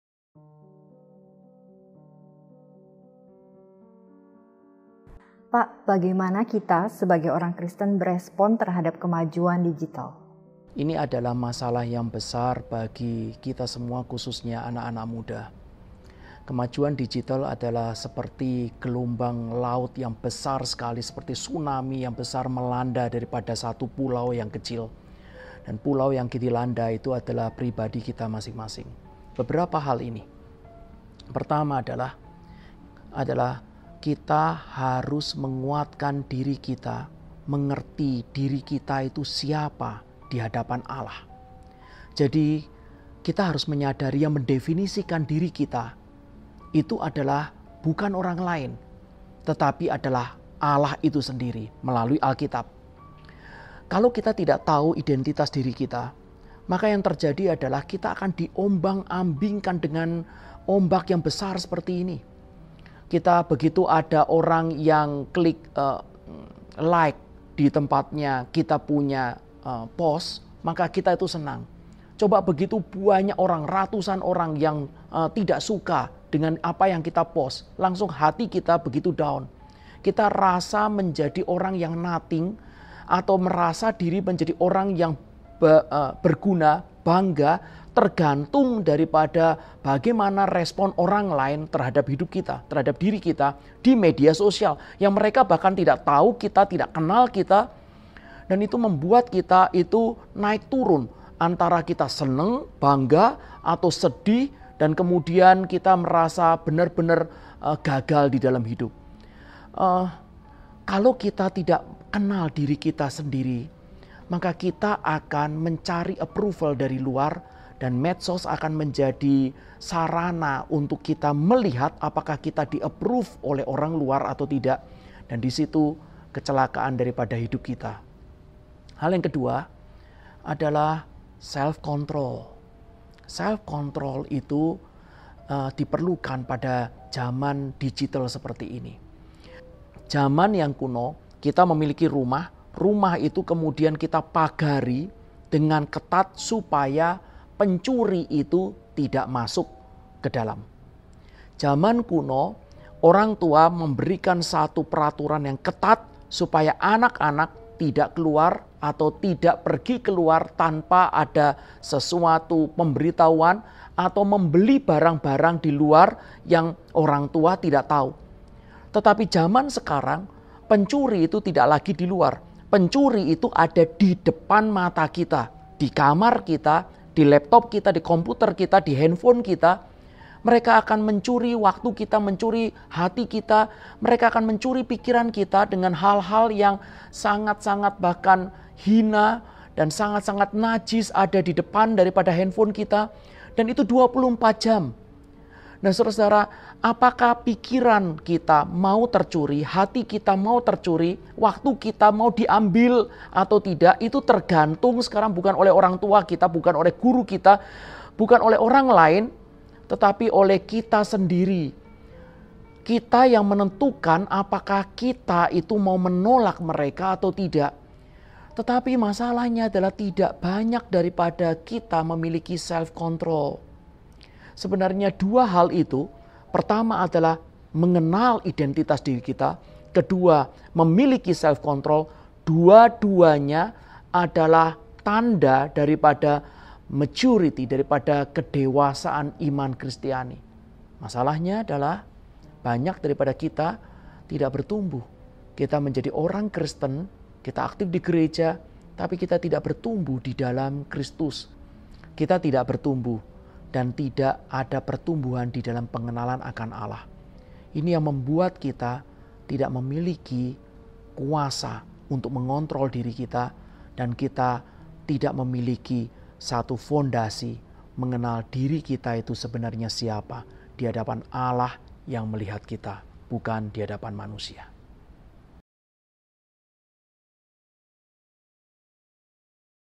Bagaimana kita berespon terhadap perkembangan dunia digital?
Kemajuan digital sudah seperti gelombang laut yang besar sekali, seperti Tsunami yang besar melanda daripada satu pulau yang kecil. Dan pulau yang dilanda itu adalah pribadi kita masing-masing. Lalu bagaimana kita harus menanggapi perkembangan dunia digital yang terjadi?

Saksikan video Q&A bersama Pdt. Agus Marjanto ini untuk mendapatkan jawaban selengkapnya.